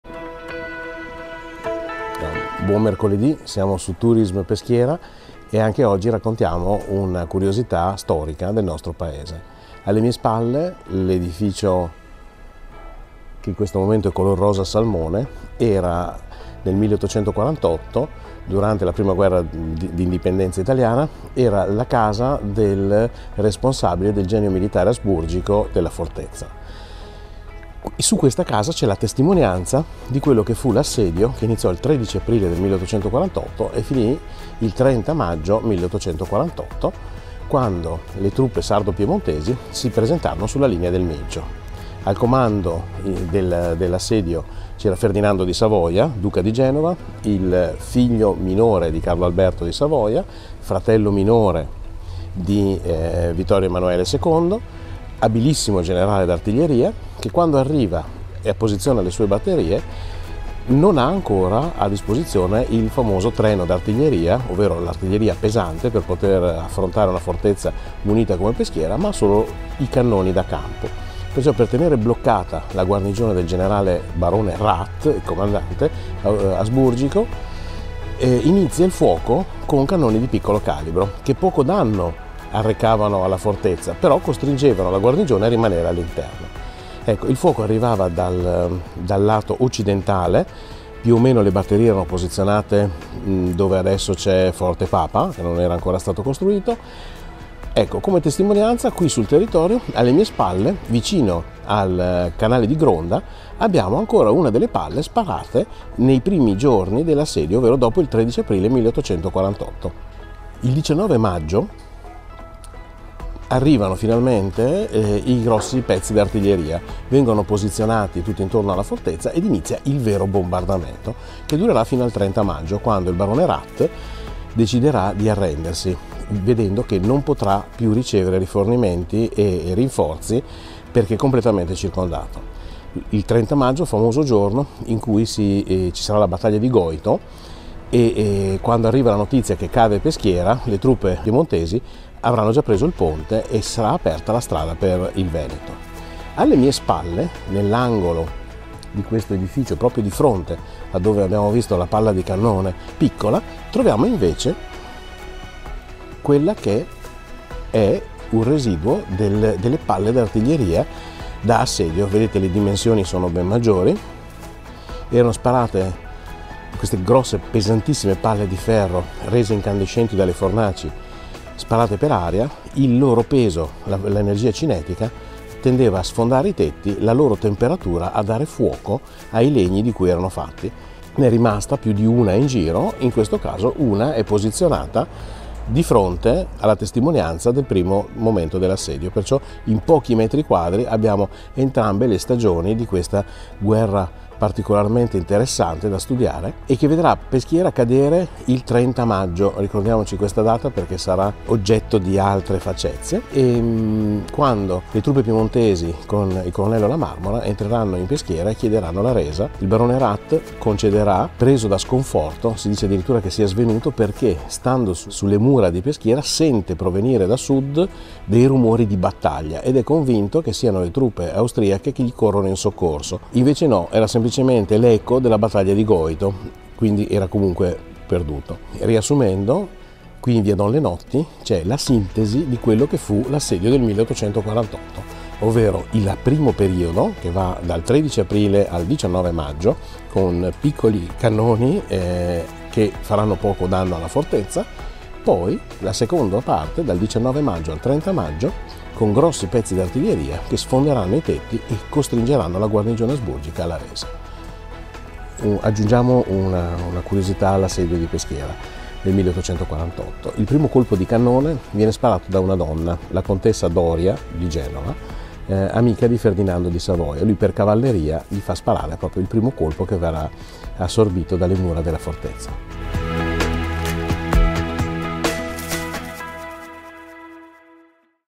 "...a Peschiera è successo il 48"
Tra le nostre vie si nasconde una testimonianza concreta delle vicende che sconvolsero l Italia e l Europa nel 1848.
Andiamo a scoprirle assieme.